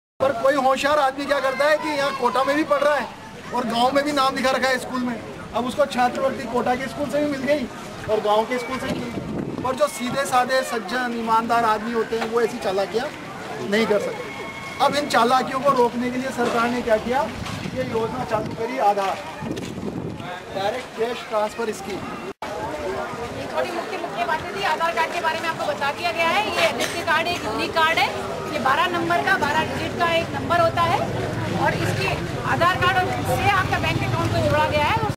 DFP Kota Spl. programme on DBT
Video clipping  DBT programme at village- Rangpur,Dist.-Kota